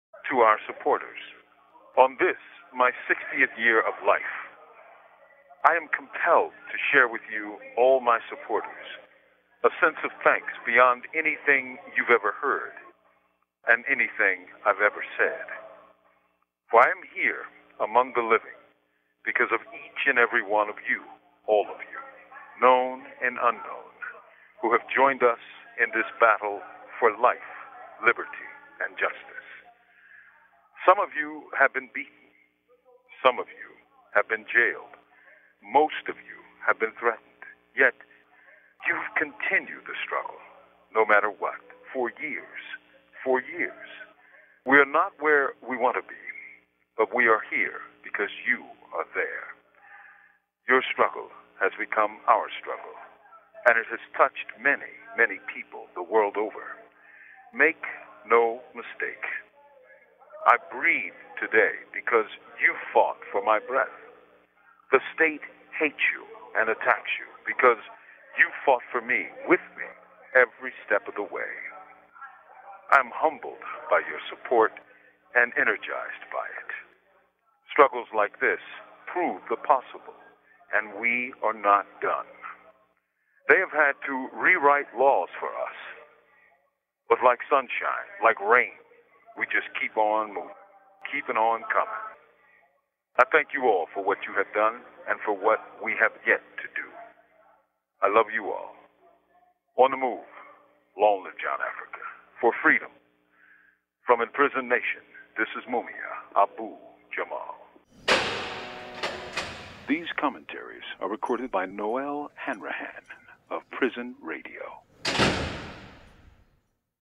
Mumia Abu-Jamal - To our supporters
This is an audio-recording of a message by Mumia Abu-Jamal to his supporters, on his 60th birthsday. Freedom for Mumia and all political prisoners!

4/24/14 - To Our Supporters

On this
my sixtieth year of life

I am compelled to share with you

all my supporters

a sense of thanks beyond anything you have ever heard

and anything I have ever said

For I am here               among the living

because of each and every one of you

All of you        known and unknown

who have joined us in this battle for life liberty and justice

Some of you have been beaten

Some of you have been jailed

Most of you have been threatened

Yet you have continued the struggle no matter what for years          for years

We are not where we want to be

But we are here because you are there

Your struggle has become our struggle

and it has touched many many people the world over

Make no mistake

I breathe today because you fought for my breath

The state hates you and attacks you because you fought for me

with me every step of the way

I am humbled by your support and energized by it

Struggles like this prove the possible

And we are not done

They have had to rewrite laws for us

But like sunshine         like rain

we just keep on moving

keeping on coming

I thank you all for what you have done and what we have yet to do

Long live John Africa

For freedom

From in prison nation this is Mumia Abu Jamal